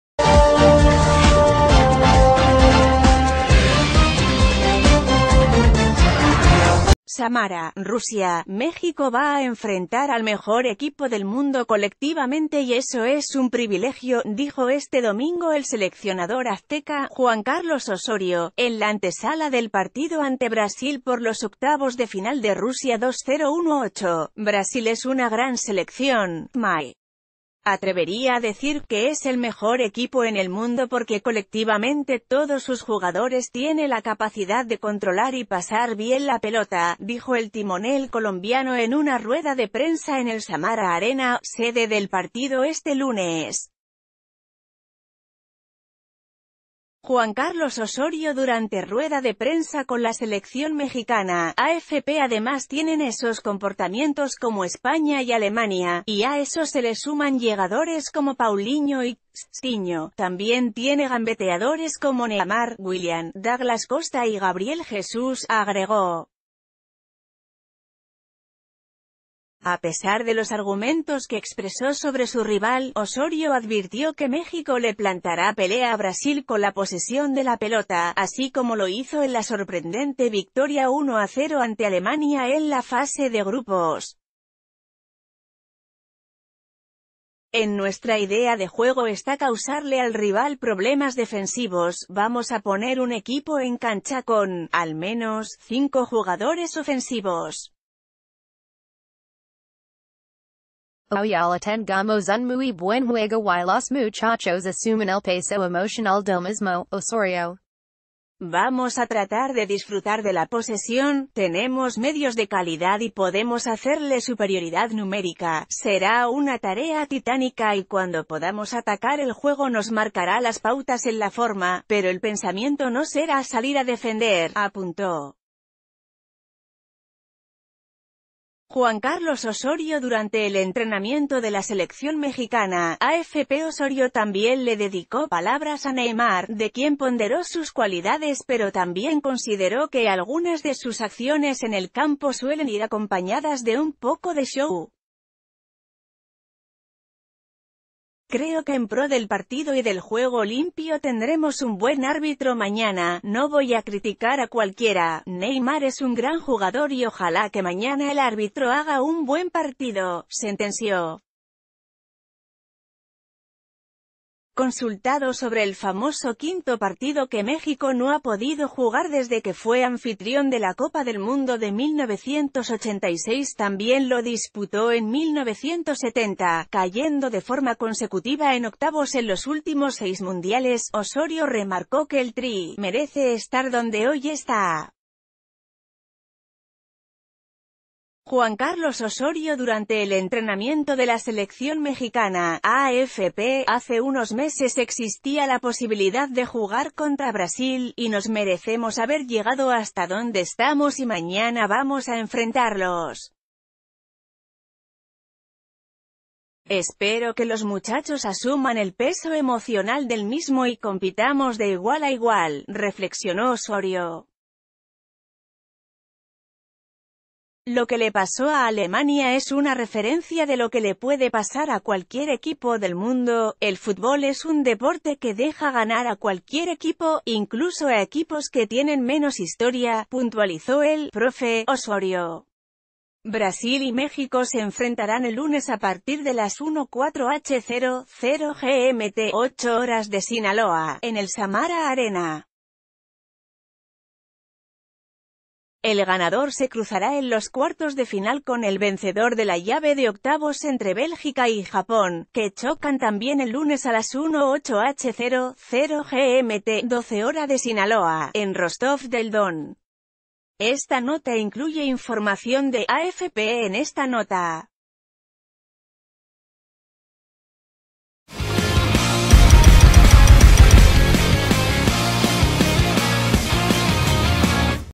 Osorio endulza a Brasil y promete un México peleando "de igual a igual"
Samara, Rusia.- México va a enfrentar al mejor equipo del mundo colectivamente y eso es un privilegio, dijo este domingo el seleccionador azteca, Juan Carlos Osorio, en la antesala del partido ante Brasil por los octavos de final de Rusia-2018.“Brasil es una gran selección, me atrevería a decir que ...